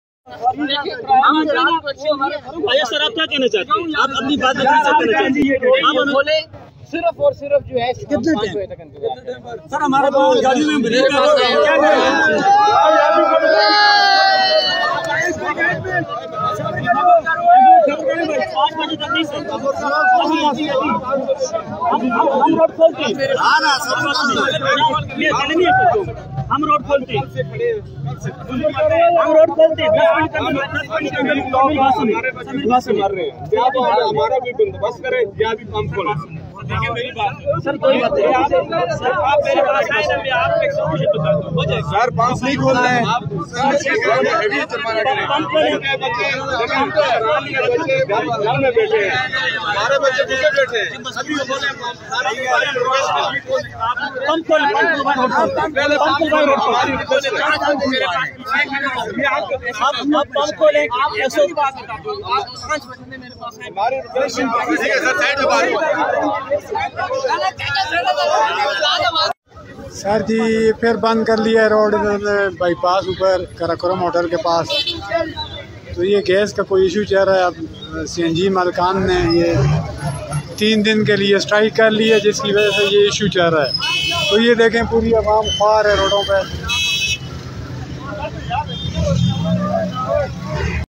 Mansehra CNG stations on strike Mansehra citizens block road against CNG mafia blackmail
MansehraNewsDigital